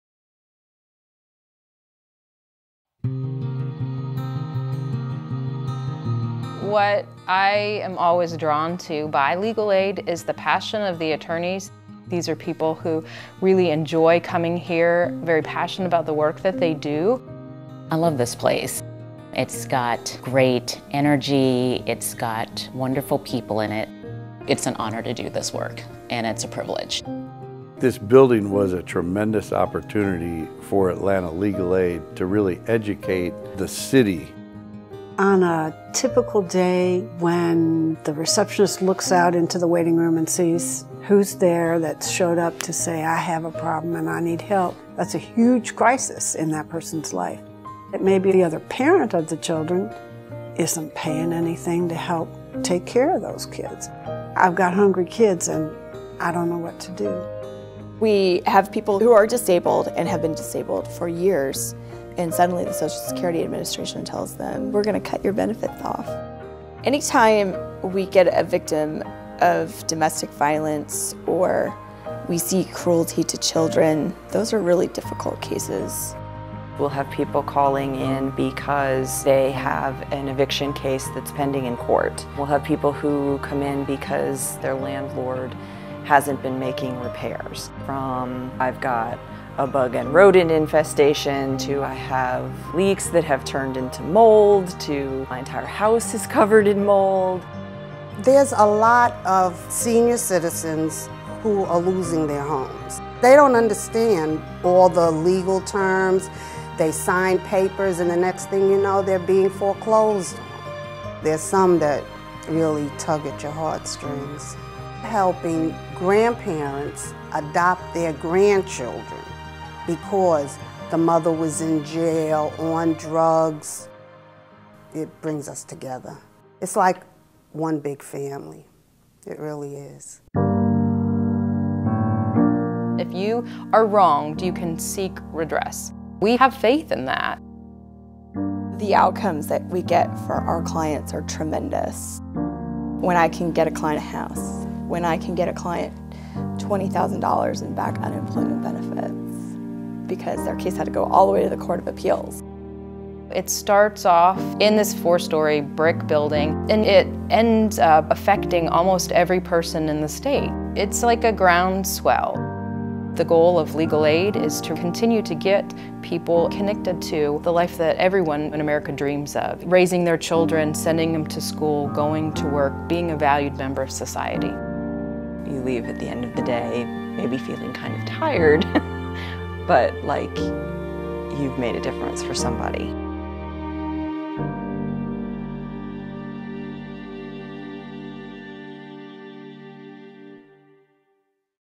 Atlanta Legal Aid Society - A Day in the Life
The Atlanta Legal Aid Society is a vital and storied institution, now headquartered at 54 Ellis St in downtown.  Legal Aid provides civil lawyers to citizens in need who cannot afford legal help.  Since 1924, they have saved people from eviction, secured healthcare for children, assisted victims of domestic violence, and successfully argued before the Supreme Court that citizens with developmental disabilities cannot be locked away in institutions.  They are the ultimate unsung heroes.